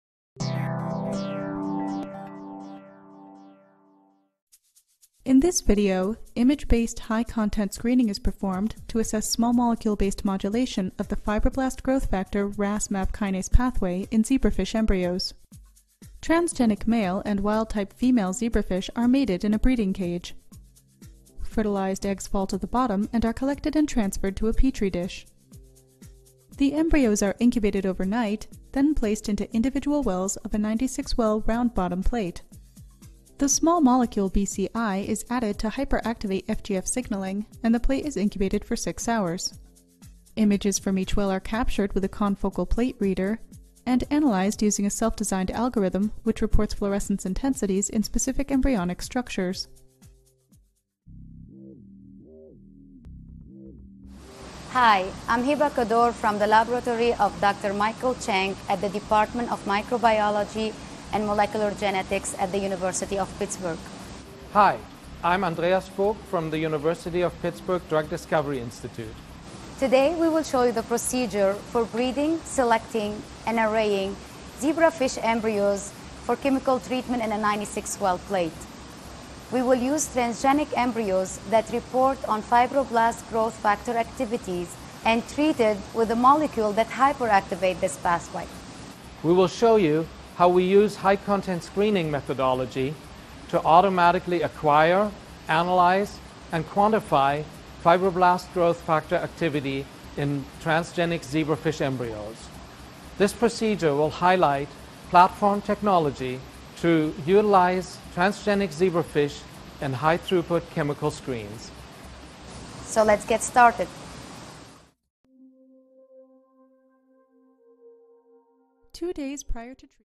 Development: Automated Imaging & Analysis For Zebrafish Chemical Screens l Protocol Preview
Development of automated imaging and analysis for zebrafish chemical screens.  -  a 2 minute Preview of the Experimental Protocol

Andreas Vogt, Hiba Codore, Billy W. Day, Neil A. Hukriede, Michael Tsang
University of Pittsburgh Drug Discovery Institute, Pharmacology and Chemical Biology; University of Pittsburgh, Department of Microbiology and Molecular Genetics; University of Pittsburgh, Department of Pharmaceutical Sciences; University of Pittsburgh, Department of Chemistry;

We report the development of a system for automated imaging and analysis of zebrafish transgenic embryos in multiwell plates. This demonstrates the ability to measure dose dependent effects of a small molecule, BCI, on Fibroblast Growth Factor reporter gene expression and provide technology for establishing high-throughput zebrafish chemical screens.